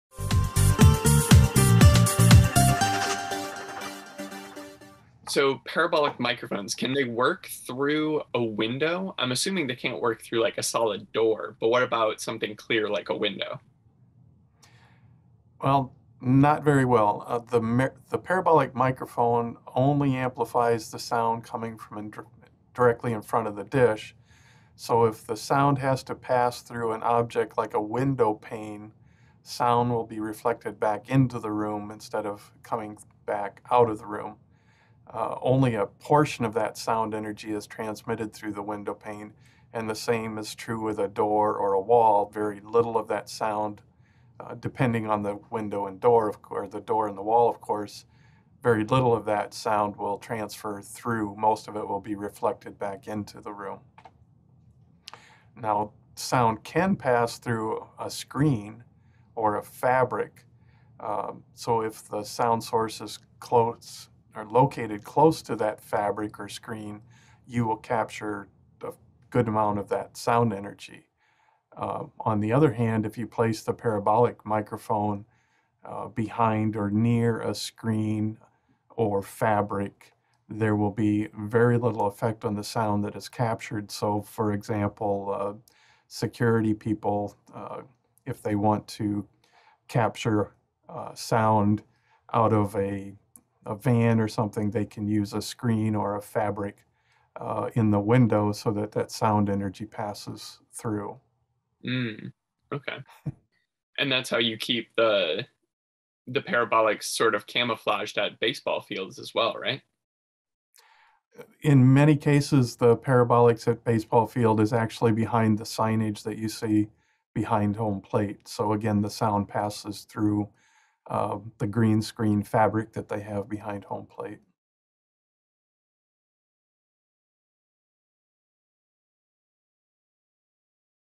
Can A Parabolic Mic Work through a Window, Door, or Screen
Do parabolic microphones work through windows? When can they hear through a screen and when can’t they? Today we’ll answer these questions for a better understanding of parabolic mic technology.